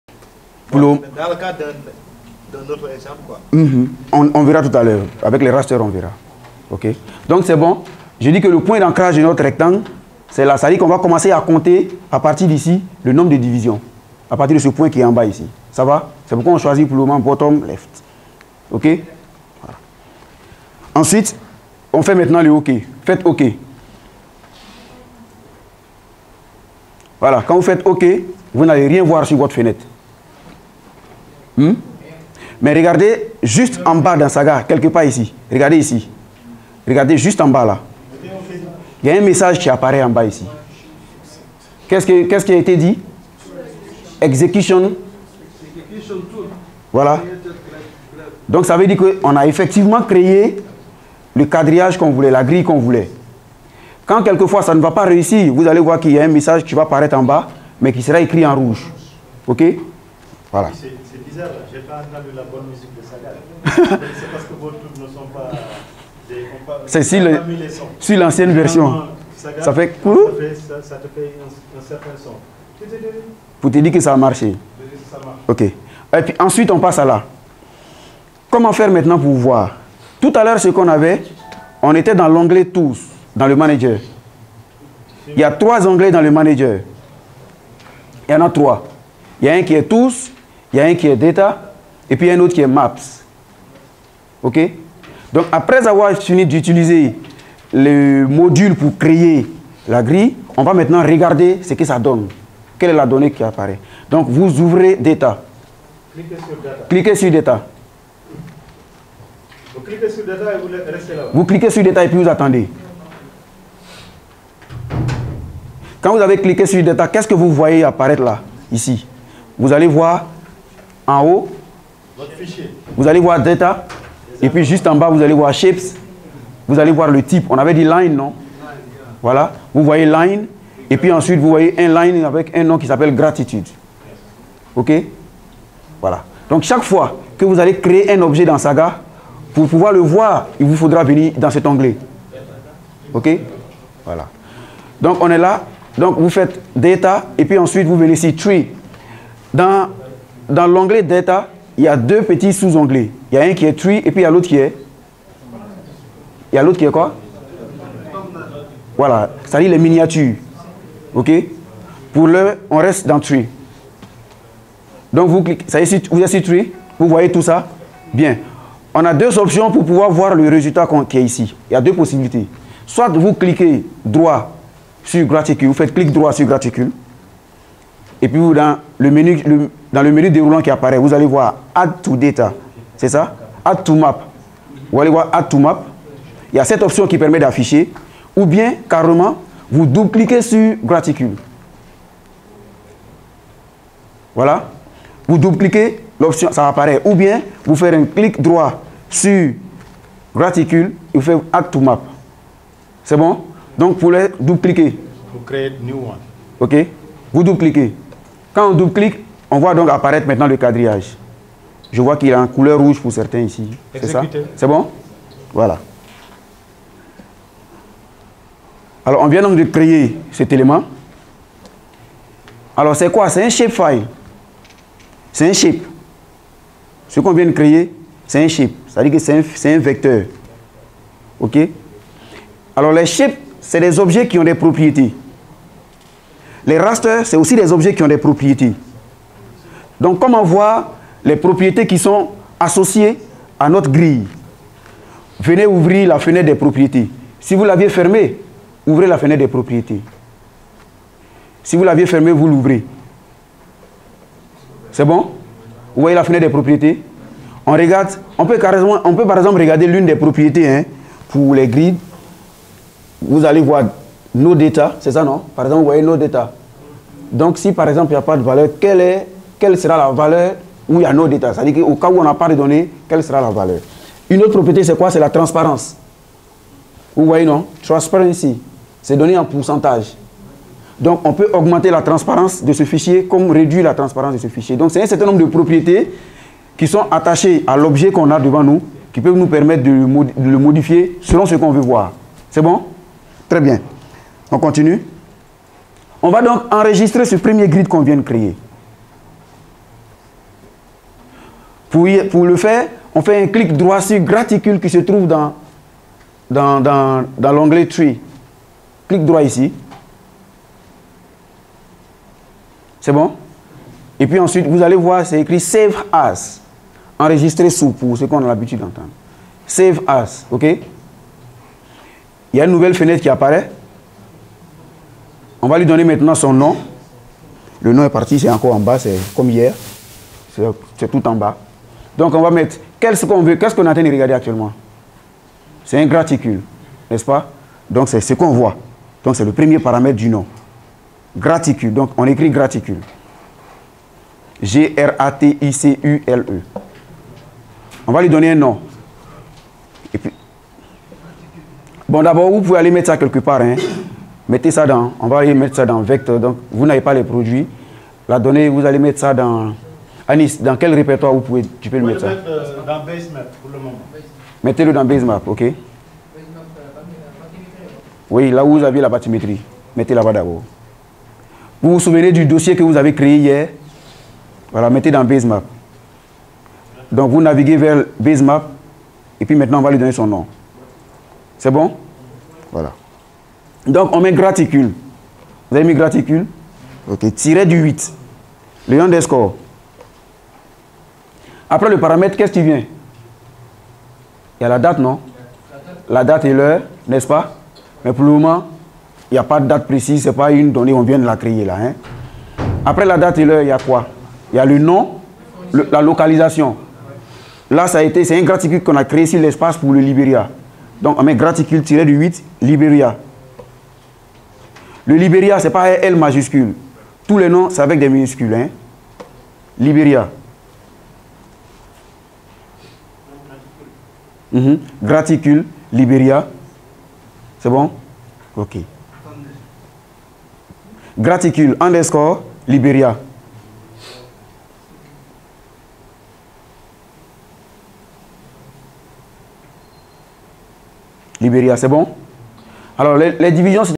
OceanTeacher - Introduction sur les fondamentaux de la gestion des données océanographiques.
Lecture by Benjamin N’Guessan (CRO, Abidjan, Côte d’Ivoire) given during the oceanographic data management training course, Dakar, Senegal, 25 - 29 Januari 2016.